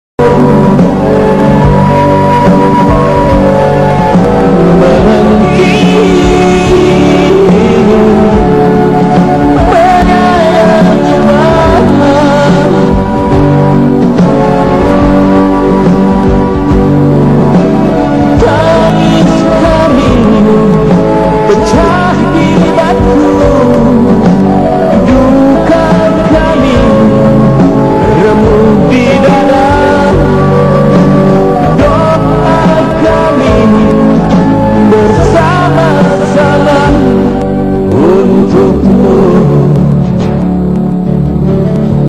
evacuate victim of porong
explorating by lapindo cause huge human dissaster in porong eastjava indonesia. Thats begun in banjar panji one at 29 mei 2006. Thats effect with enviroment damage and in 31 juli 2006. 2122 family or 8146 people was evacuated